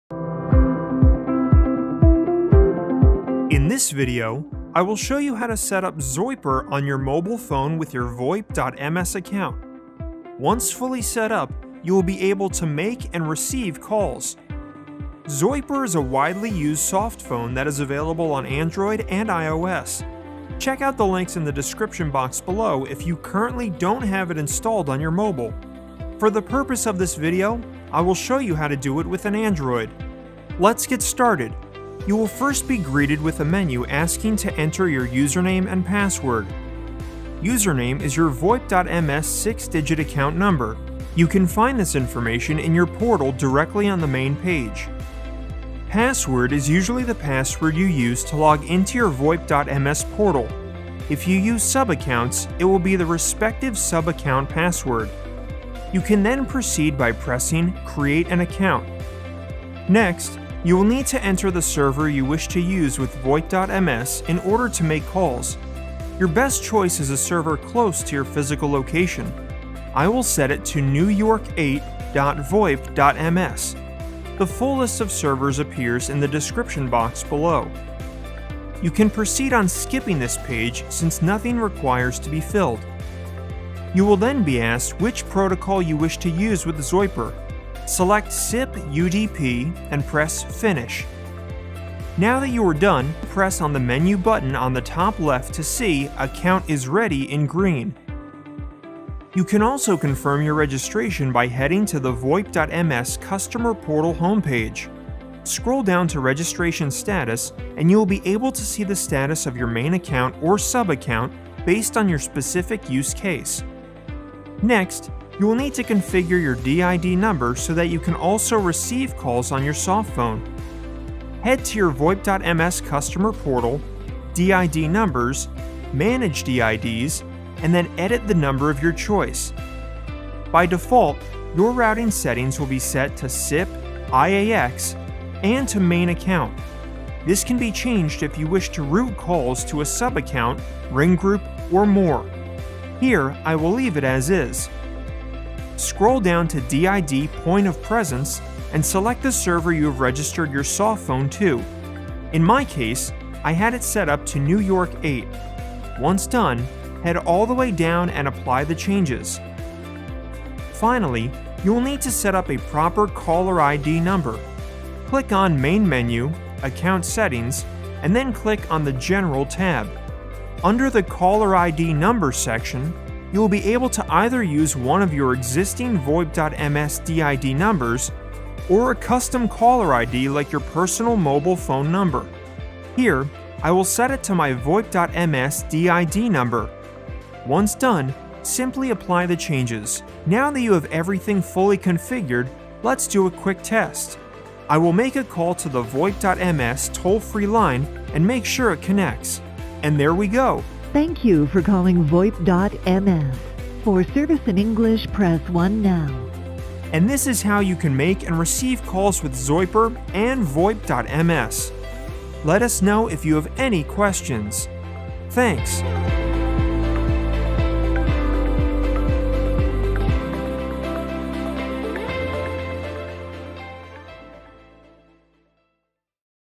How to Set Up Zoiper with VoIP.ms
Here is our tutorial video on How to Set Up Zoiper with your VoIP.ms account!

To set up your own Zoiper softphone, follow the step-by-step guide in our Wiki

Intro & About Zoiper - 0:00
Account setup on Zoiper - 0:30
Confirm registration - 1:31
Configure your number for your softphone - 1:55
Test your setup - 3:10